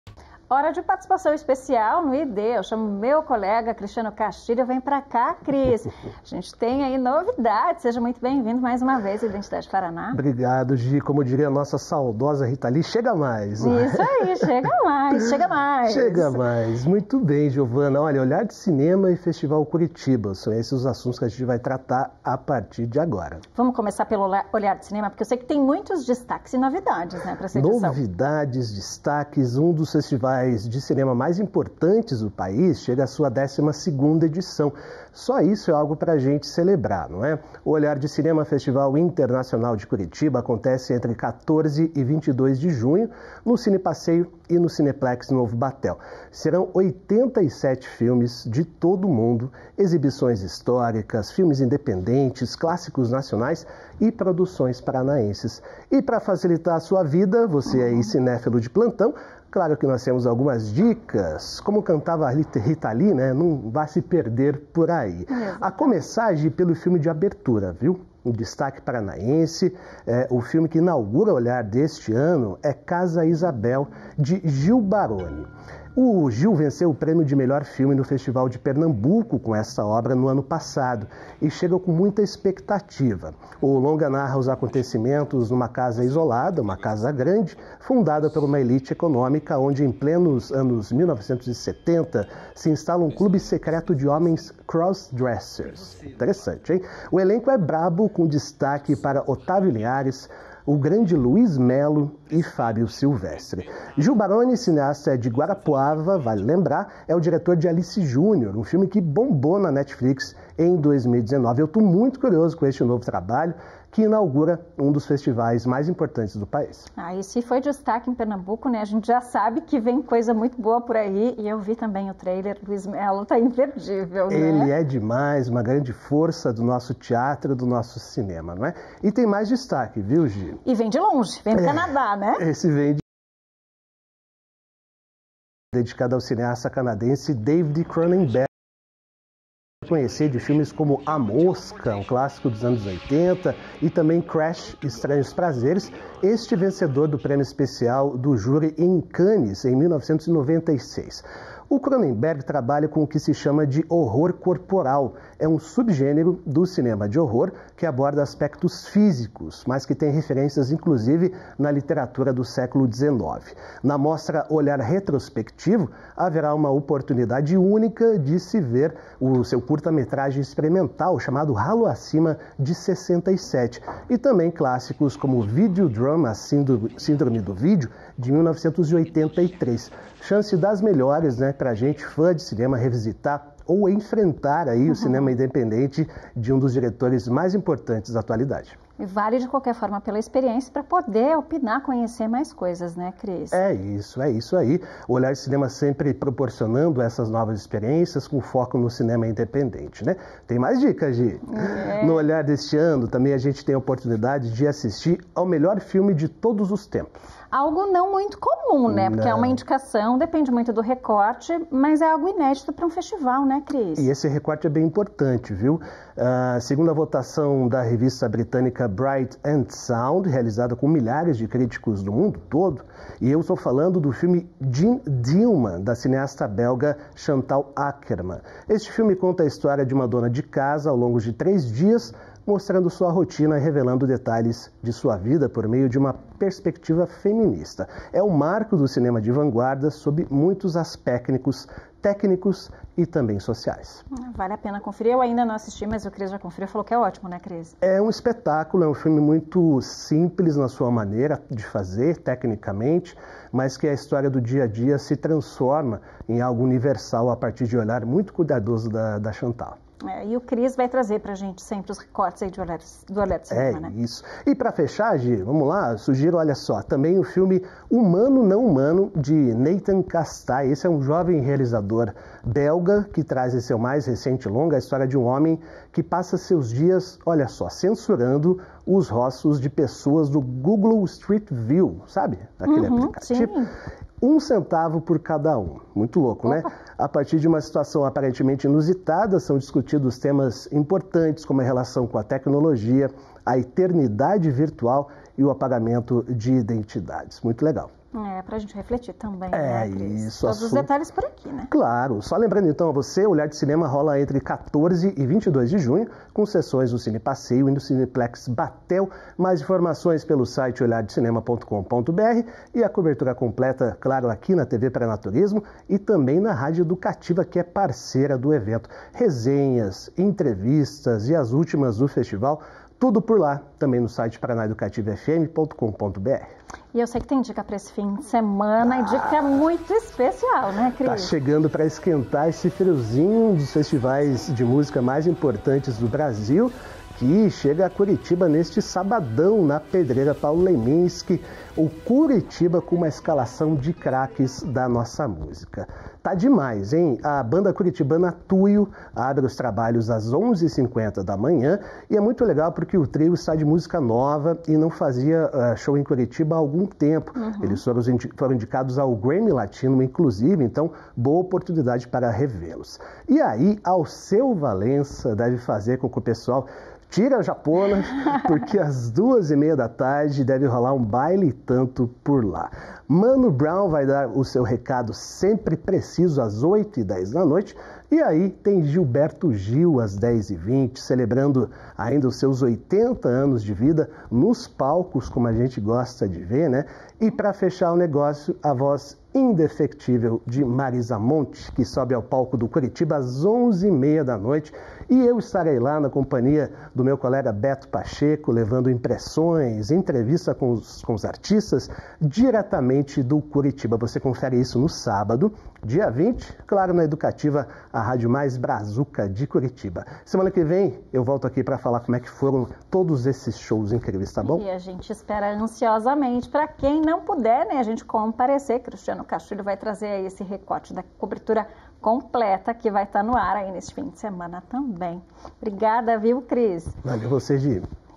Coluna ID com Cris Castilho
Hora de participação especial no ID. O jornalista Cristiano Castilho traz dicas do 12º Olhar de Cinema, que será em junho. E, ainda, as atrações do Coolritiba, que promete movimentar o fim de semana na capital.

🔸 Sintonize nossa programação no canal 9.1 UHF, no canal 509 da NET e na antena parabólica pelo satélite Star One D2.